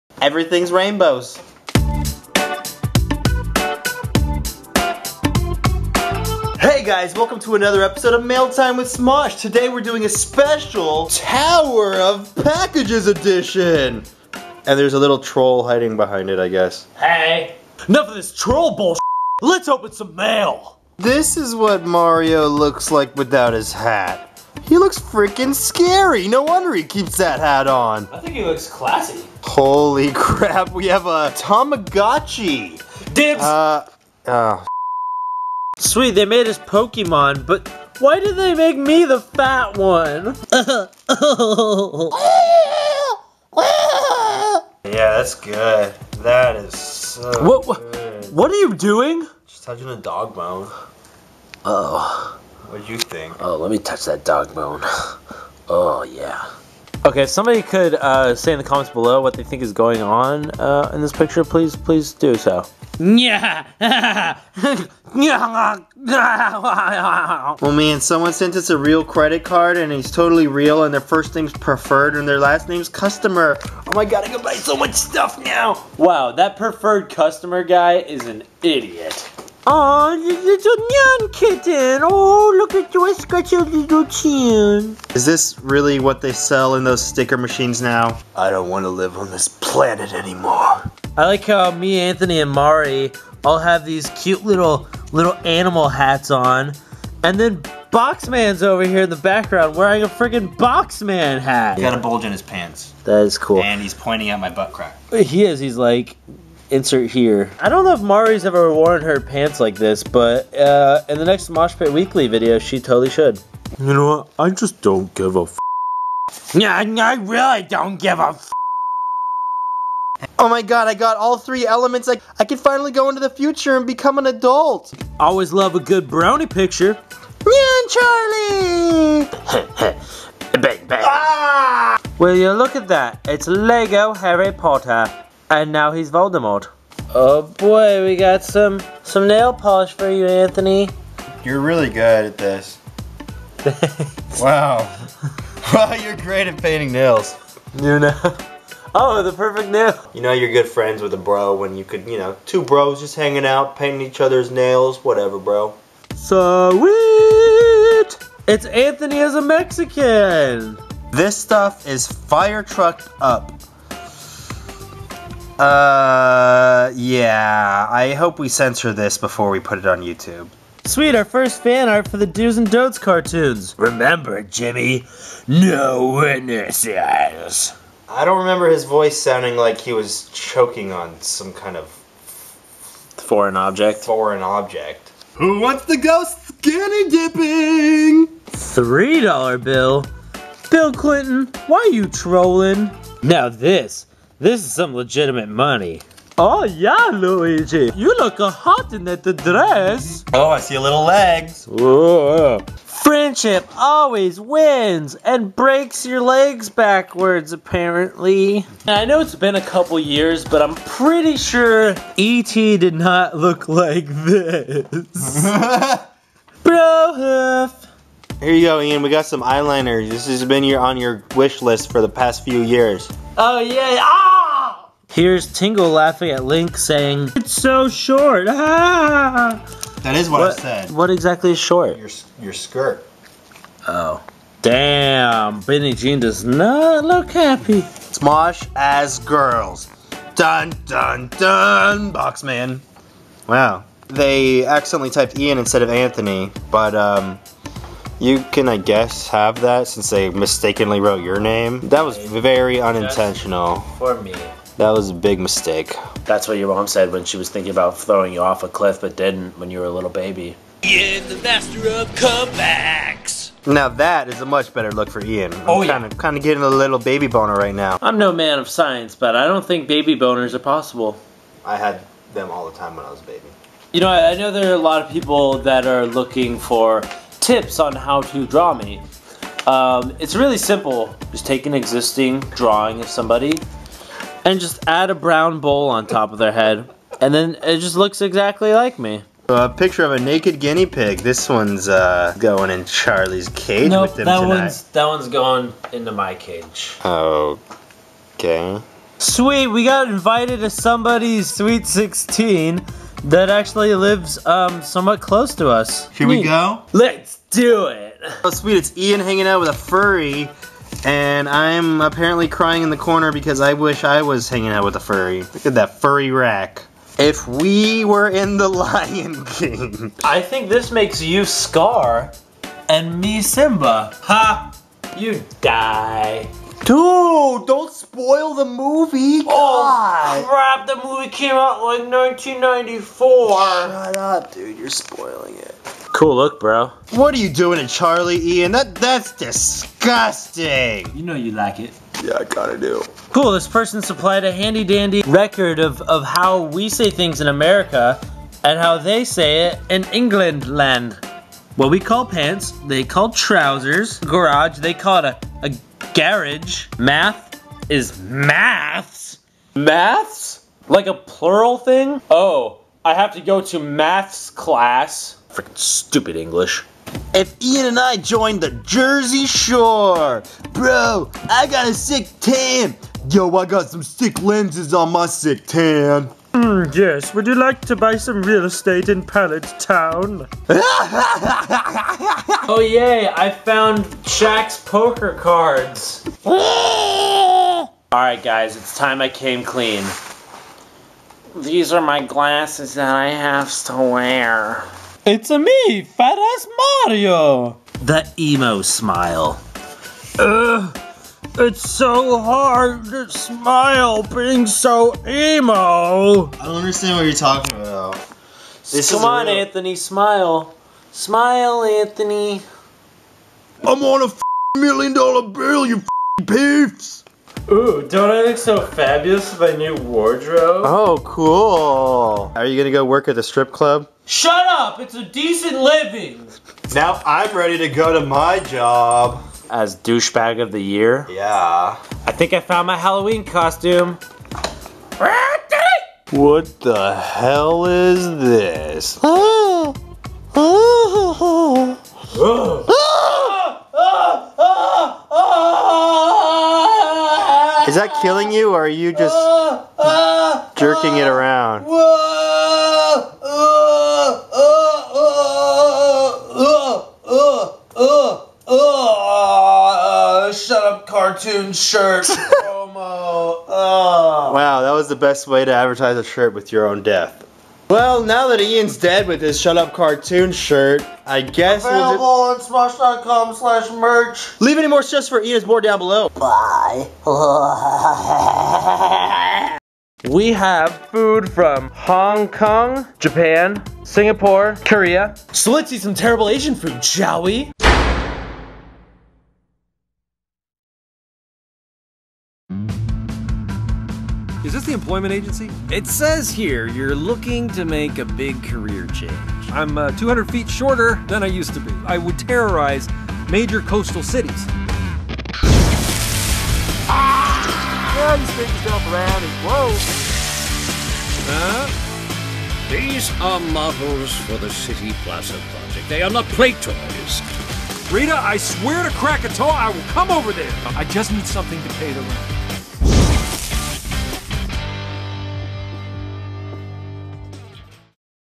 STRIPPER CLOTHES IN OUR MAIL?!
Sexy dresses,Inappropriate drawings, and bros painting each other's nails, it's MAILTIME!